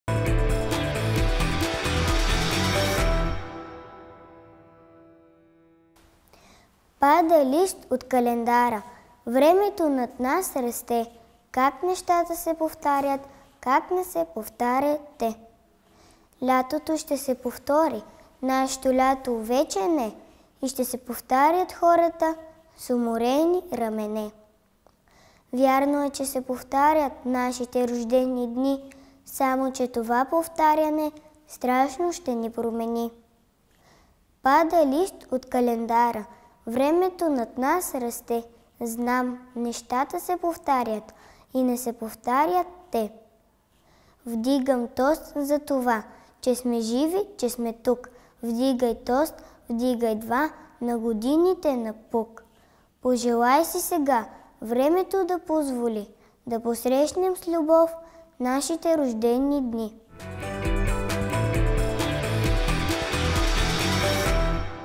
"Родолюбие" - „Рожден ден“ от Петър Караангов
В традиционната ни рубриката „Родолюбие“ представяме стихотворението „Рожден ден“ от Петър Караангов. Рецитира Яна Кочева Сандански.